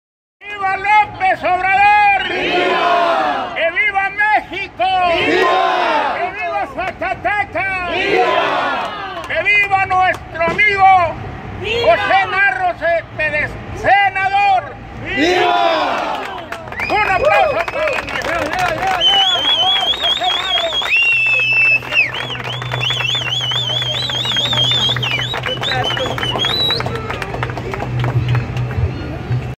José Narro Céspedes estará al servicio y trabajará por Zacatecas
Durante su festejo como virtual ganador de la elección a Senador de la República, José Narro Céspedes afirmó que estará al servicio y trabajará por Zacatecas.